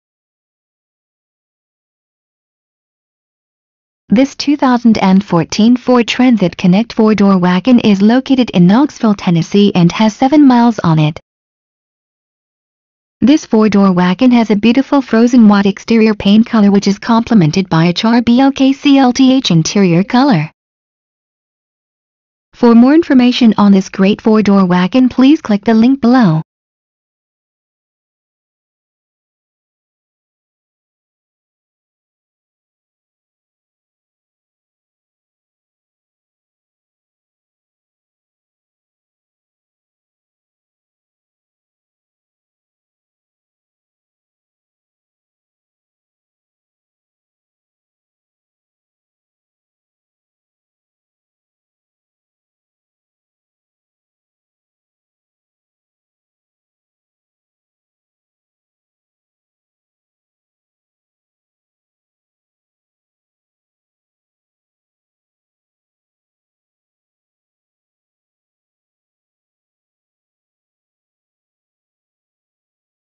2014 Ford Transit Connect Knoxville TN
Price: $ 24,777
Mileage: 7
Trim: XLT
Body Style: 4D Wagon
Ext. Color: Frozen White
Int. Color: Char Blk Clth
Engine: 2.5L
Drive Type: FWD
Fuel Type: Gasoline
Doors:
Stock No.: 40635
VIN: NM0GE9F70E1139388